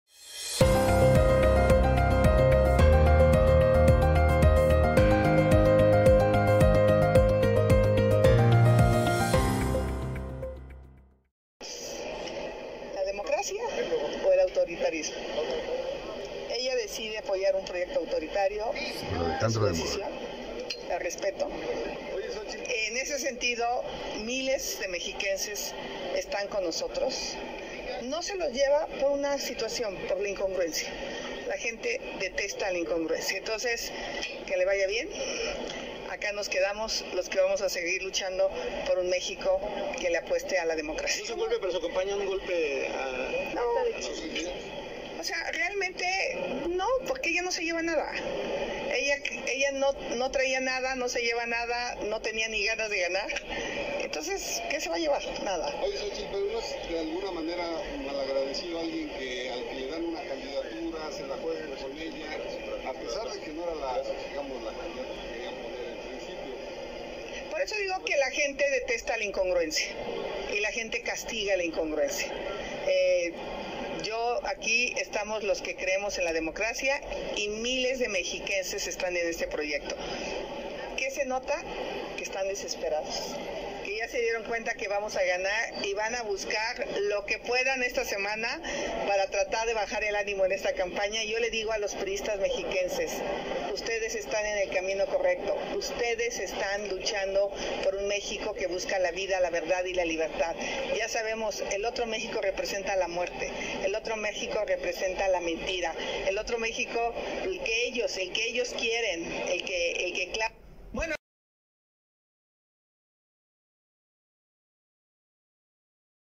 Xóchitl Gálvez critica a Alejandra del Moral por apoyar a Claudia Sheinbaum. Quien renunció al Pri
Cd de México.- Xóchitl Gálvez critica a Alejandra del Moral por apoyar a Claudia Sheinbaum.
Es de recordar que Alejandra Del Moral - quien ahora renunció al Pri- había sido la candidata perdedora, en elección pasada a gobernadoras en el Edo de México.